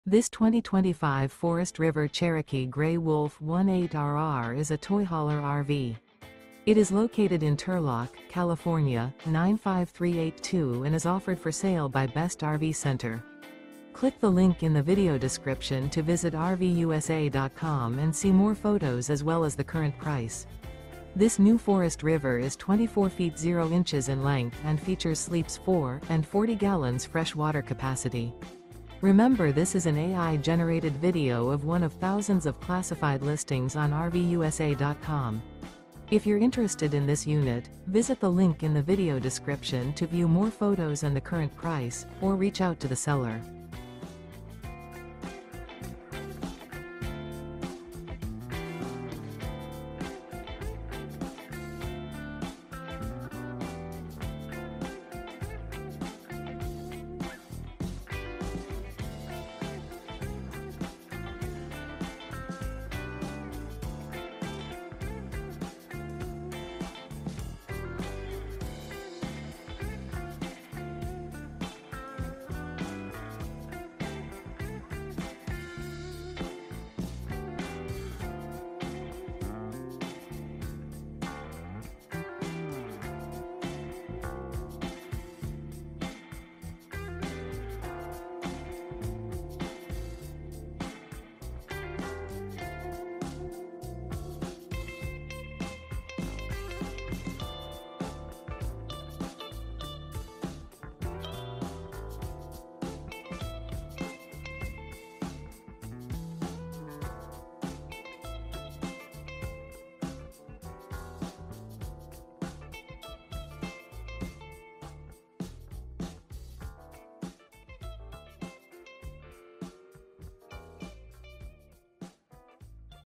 Phenomenal 2025 Forest River Cherokee Grey Wolf Toy Hauler RV For Sale in Turlock, CA | RVUSA.com
This 2025 Forest River Cherokee Grey Wolf 18RR is a Toy Hauler RV. It is located in Turlock, CA 95382 and is offered for sale by Best RV Center. This New Forest River Is 24' 0" in length and features sleeps 4, and 40 gal fresh water capacity..